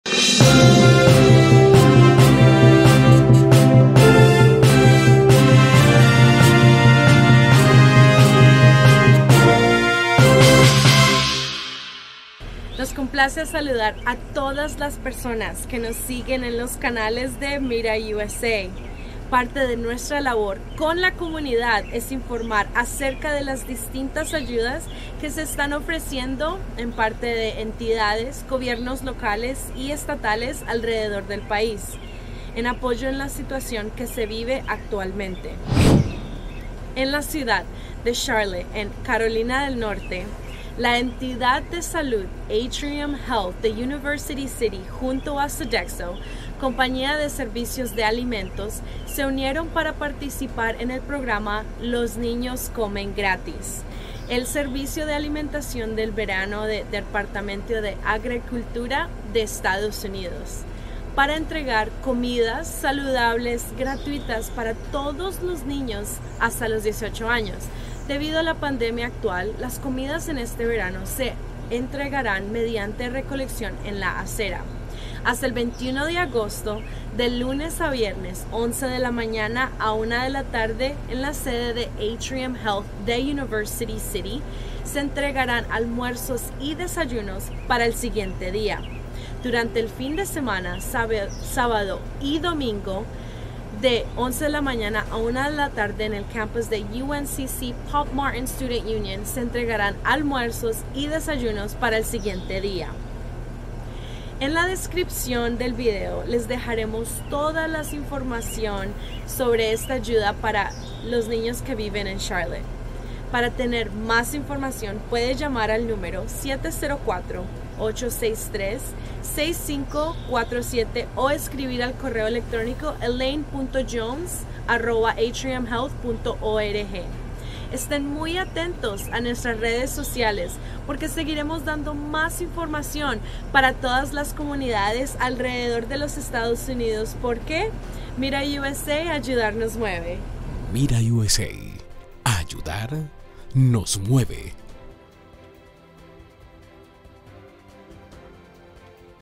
MIRA USA Informa: Comidas saludables gratuitas para todos los niños hasta los 18 años en Charlotte
En la ciudad de Charlotte, en Carolina del Norte, la entidad de salud Atrium Health de University City junto a Sodexo, compañía de servicios de alimentos, se unieron para participar en el programa: “Los niños comen gratis”, del Servicio de Alimentación de Verano del Departamento de Agricultura de Estados Unidos, para entregar comidas saludables gratuitas para todos los niños hasta los 18 años.

Debido a la pandemia actual, las comidas de este verano se entregarán mediante la recolección en la acera, hasta el 21 de agosto.

De lunes a viernes de 11:00 a.m. a 1:00 p.m. en la sede de Atrium Health de University City, se entregarán almuerzos y desayunos para el siguiente día.

Durante el fin de semana (sábados y domingos), de 11:00 a.m. a 1:00 p.m. en el campus de la UNCC Popp Martin Student Union, se entregarán almuerzos y desayunos para el siguiente día.

En la descripción del video, les dejaremos toda la información sobre esta ayuda para los niños que viven en Charlotte.